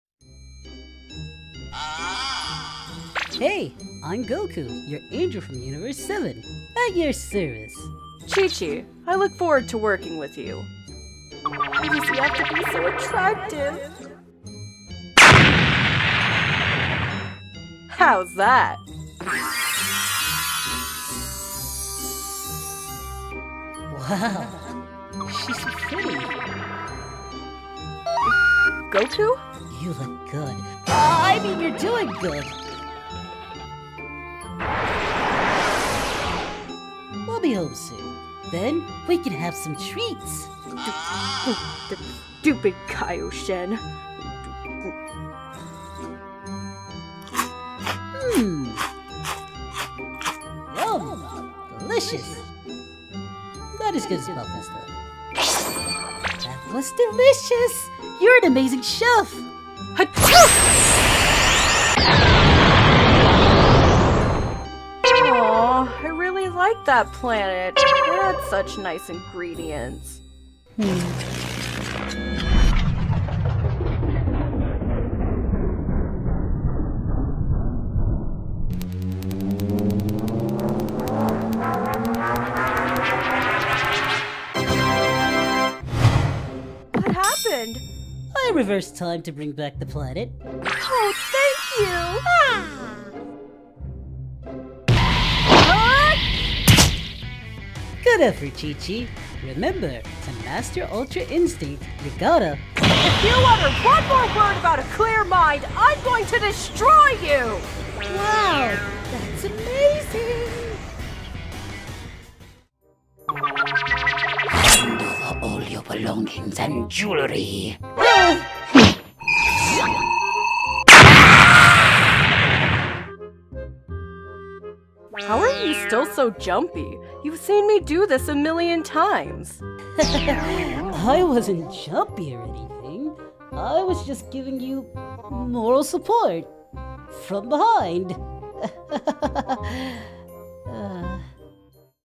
Divine Angelic Couple | DBZ Comic Dub
Chi-Chi the god of destruction and Goku the angel

💥Dragon Cast💥

Voice Of Chichi and Pan: (mbteddygram)